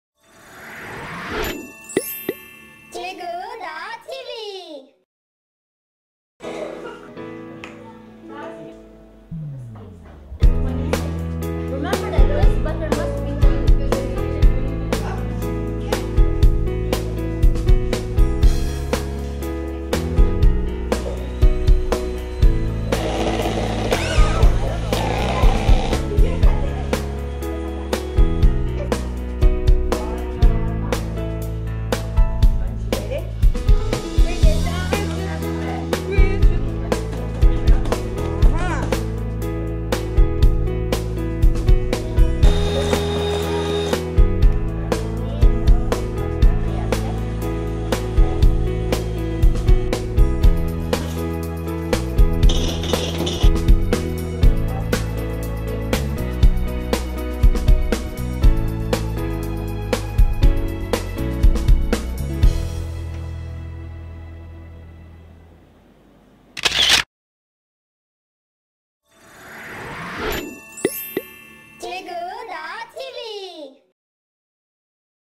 Baking with Fun Bee Park
Asides from Arts & Crafts such as batik painting and clay modelling, Fun Bee Park also provide children with further knowledge and skills with their cooking lessons.